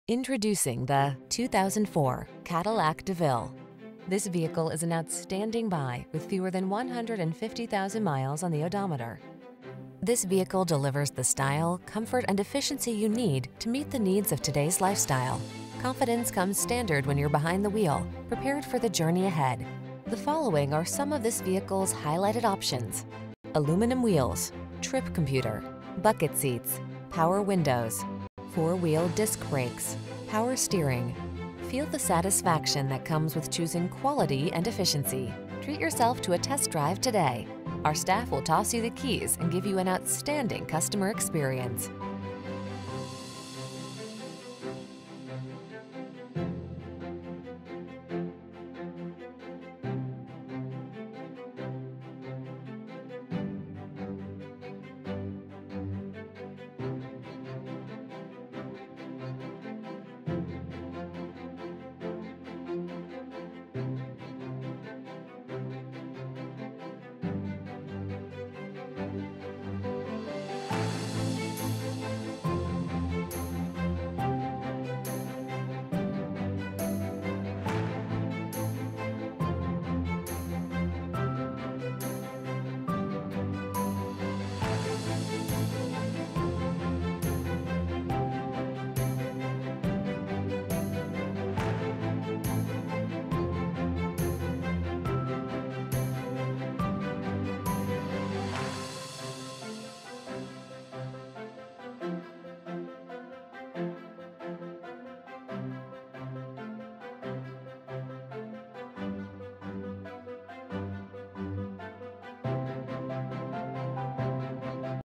2004 Cadillac DeVille New Castle, Muncie, Anderson, Fishers, Noblesville, IN 2130702
Cashmere Used 2004 Cadillac DeVille available in New Castle, Indiana at Russ Hubler CDJR. Servicing the Muncie, Anderson, Fishers, Noblesville, IN area.

Recent Arrival!brbrCashmere 2004 Cadillac DeVille FWD 4-Speed Automatic with Overdrive Northstar 4.6L V8 SFI DOHCbrbr18/26 City/Highway MPG